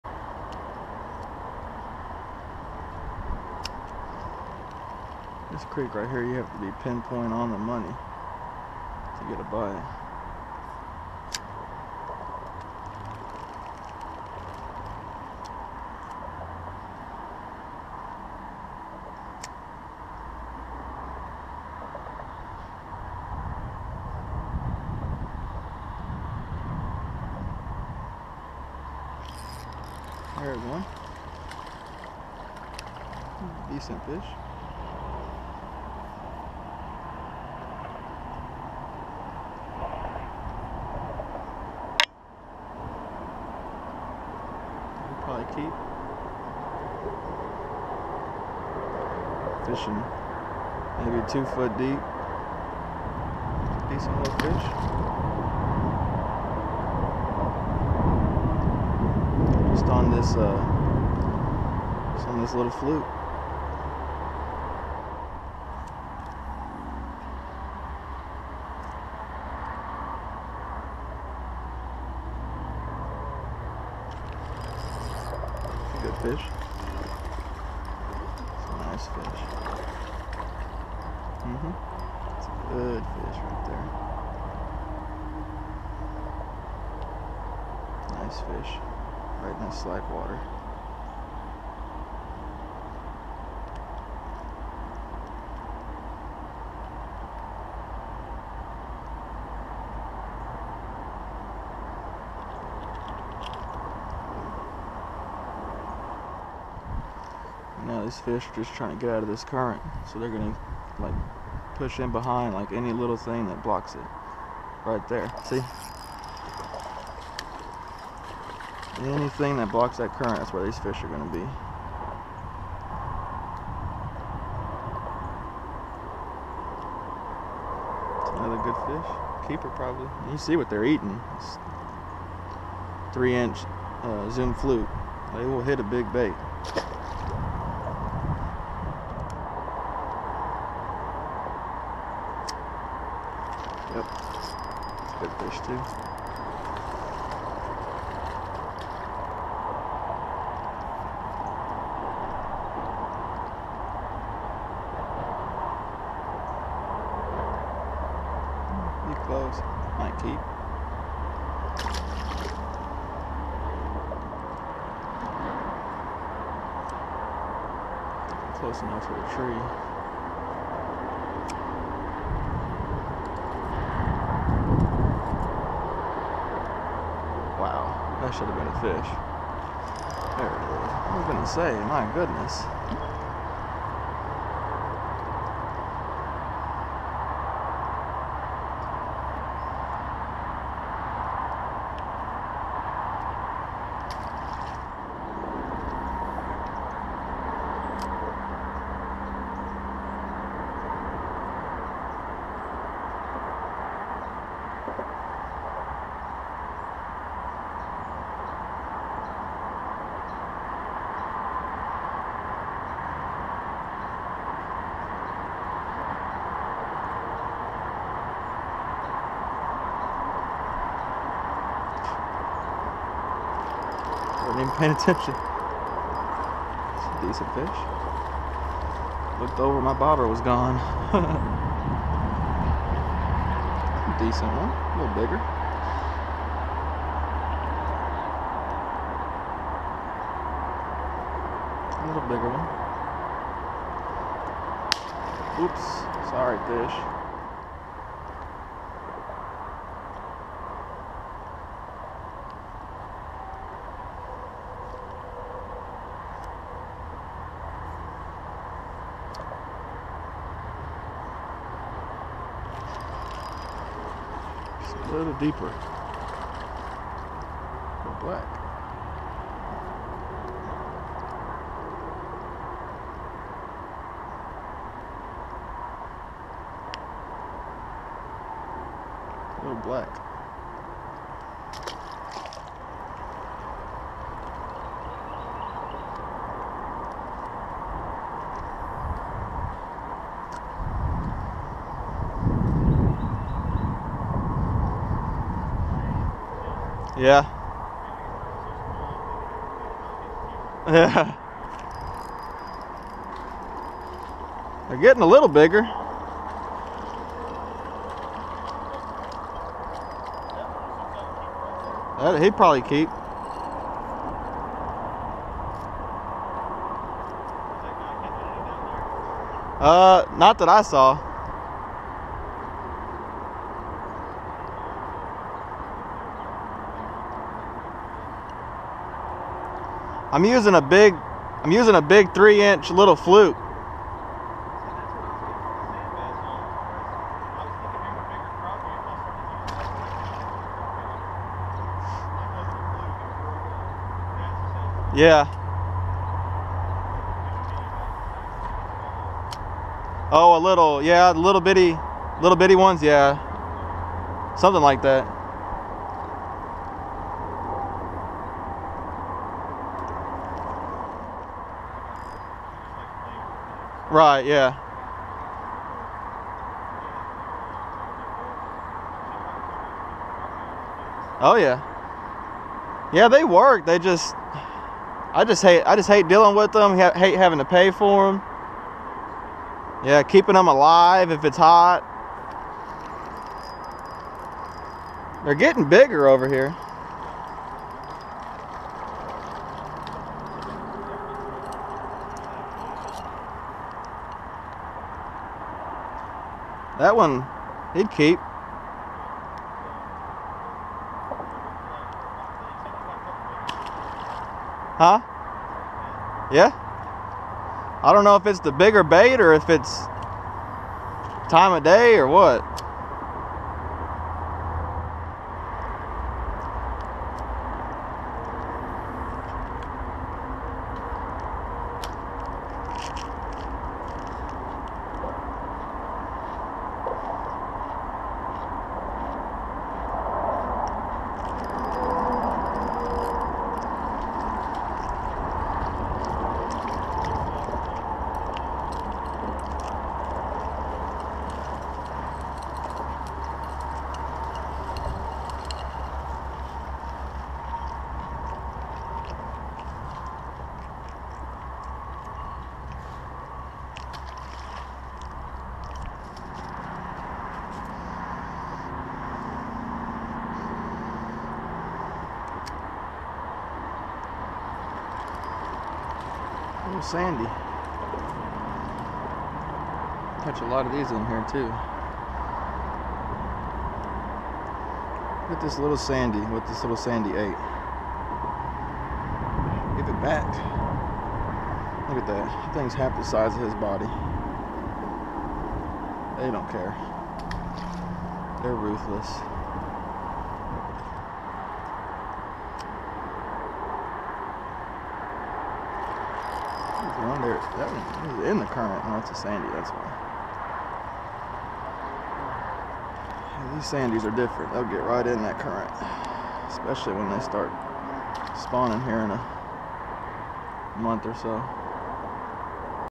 How to catch crappie in a shallow creek. What to look for?
Here is a short video from yesterday when I was enjoying some of the last good days of creek fishing for a while. (suppose to rain heavily all weekend). I was using 3 inch zoom flukes in albino shad and baby bass about 2 ft under a bobber. I was also using my 5ft 6 inch Sniper rod with 6 pound Mr. Crappie Hi-Vis line. There is nothing like a big crappie pulling your bobber under! I hope you all enjoy. I will have another creek fishing video next week with more good sized fish.